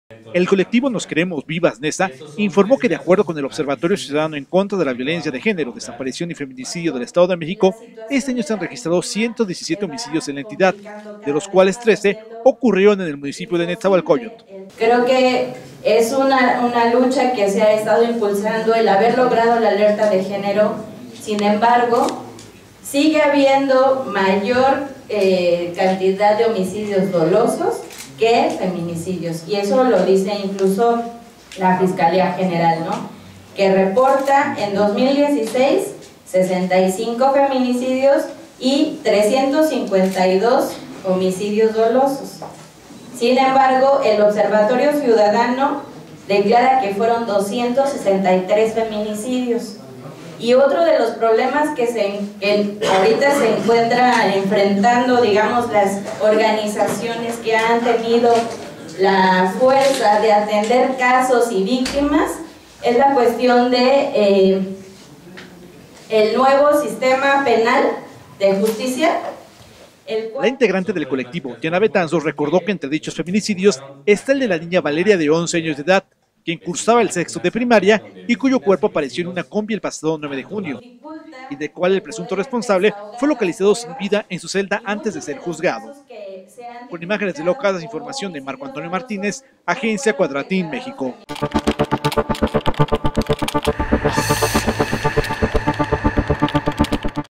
Denuncia NosQueremosVivasNeza 117 feminicidios en Edomex este año
CIUDAD DE MÉXICO, 20 de junio de 2017.- El colectivo NosQueremosVivasNeza informó que de acuerdo con el Observatorio Ciudadano en contra de la Violencia de Género, Desaparición y Feminicidio del Estado de México, este año se han registrado 117 homicidios en la entidad, de los cuales 13 ocurrieron en el municipio de Netzahualcóyotl.